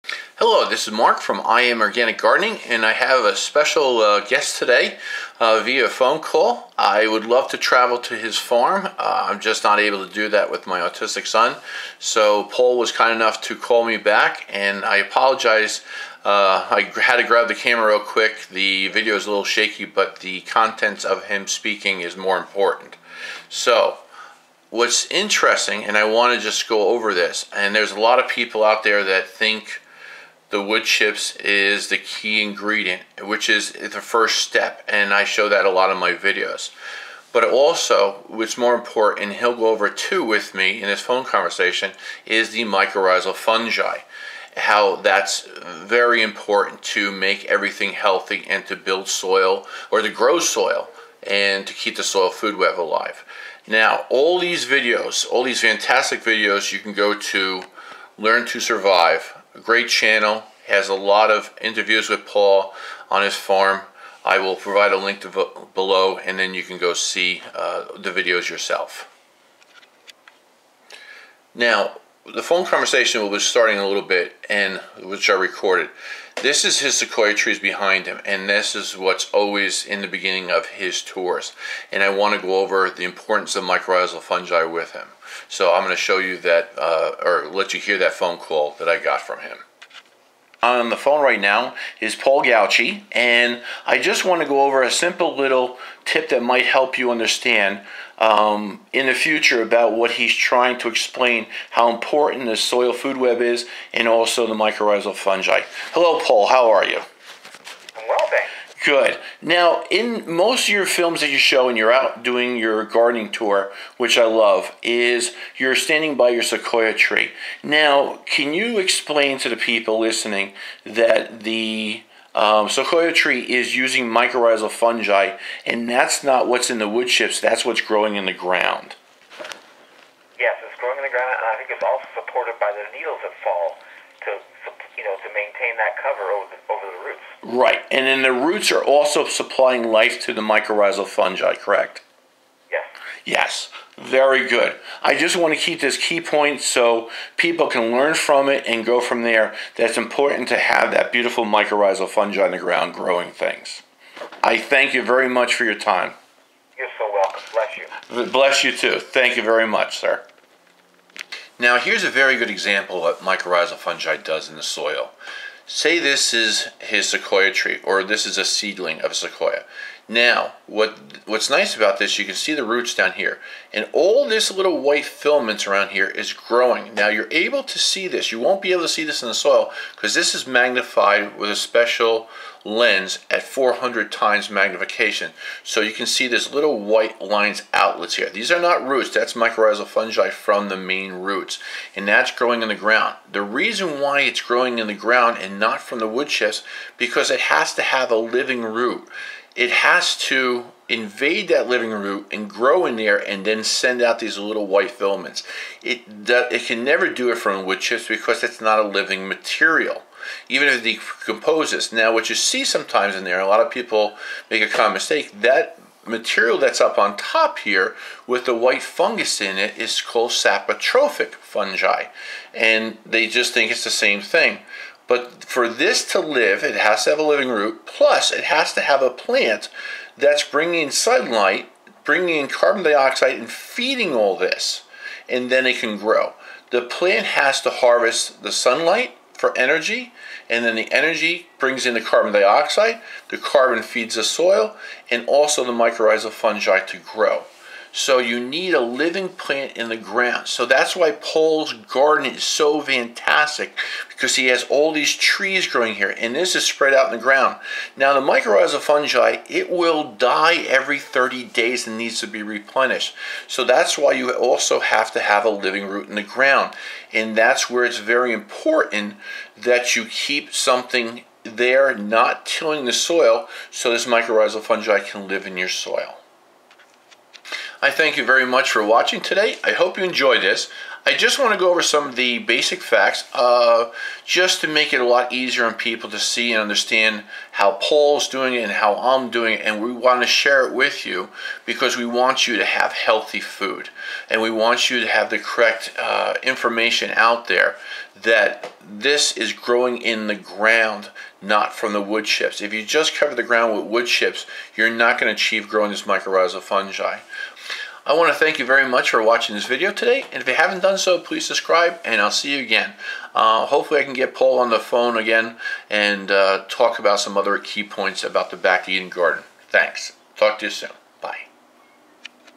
My Talk with Paul G. from Back to Eden Gardening Method with wood chips
Key fact about Fungi.. My Talk with Paul G. from Back to Eden Organic Gardening Method 101 with wood chips. Also No till vegetable garden for beginners.